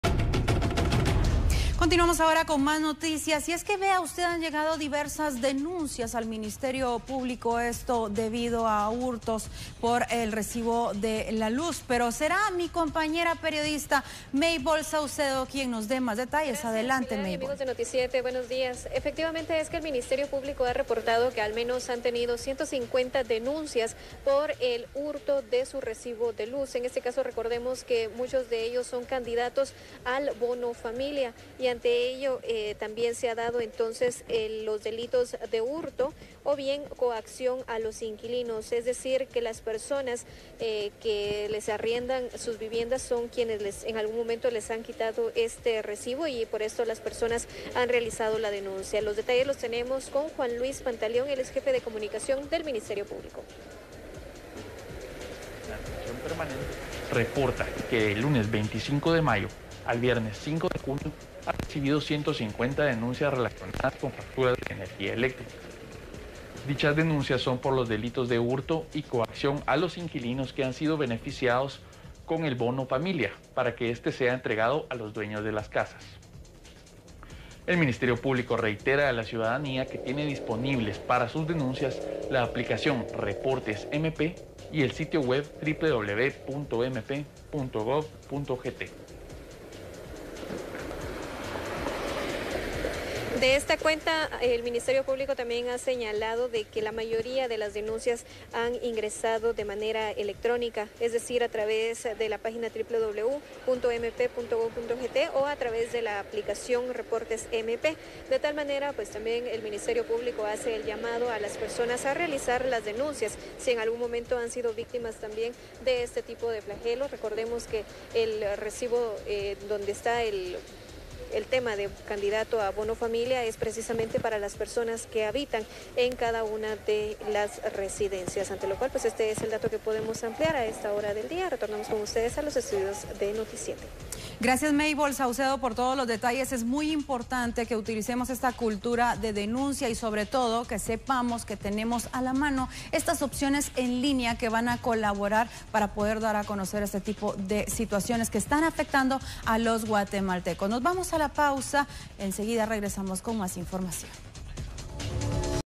150 denuncias por hurto de recibos de energía eléctrica
Diversas denuncias en el Ministerio Público por hurtos en el recibo de la energía eléctrica, se registran 150 denuncias. Esto para adjudicarse 'bono familia' del gobierno.